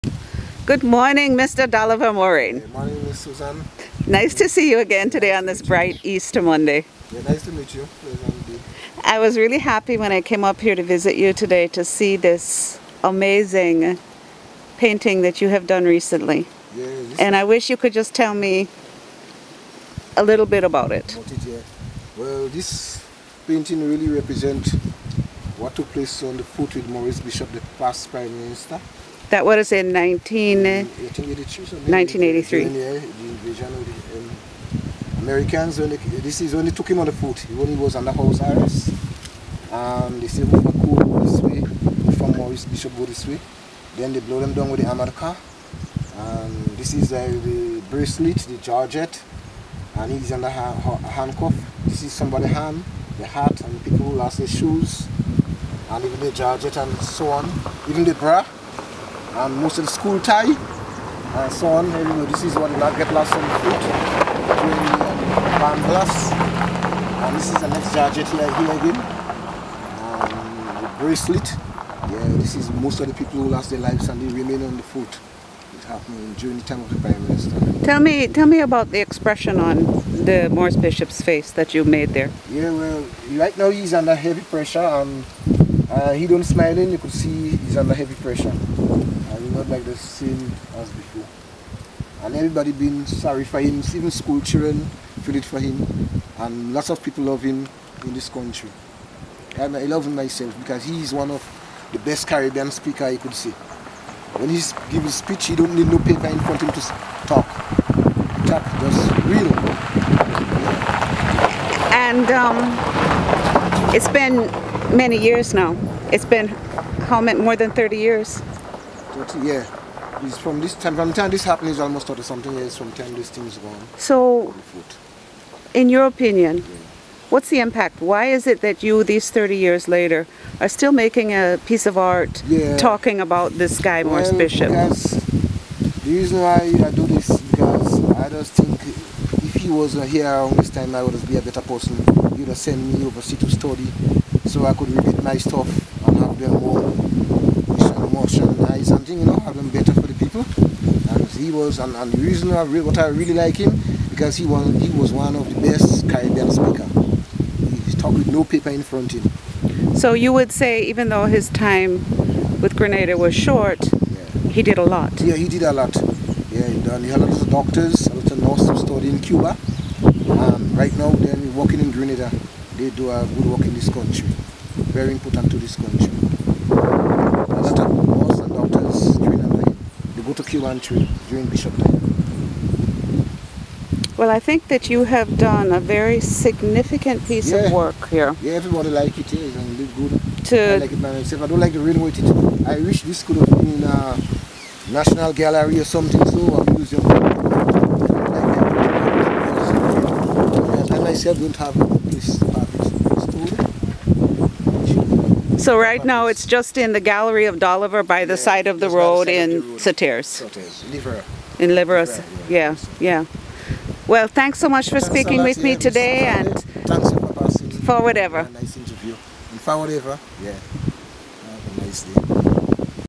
Susan Mains Gallery Contemporary Art in Grenada-- Doliver Morain
Doliver Morain talks about his recent work portraying slain Prime Minister Maurice Bishop.  Bishop was killed in 1983 during a period of unrest in Grenada's history. This intuitive artist explores the implications. Work completed in March 2016.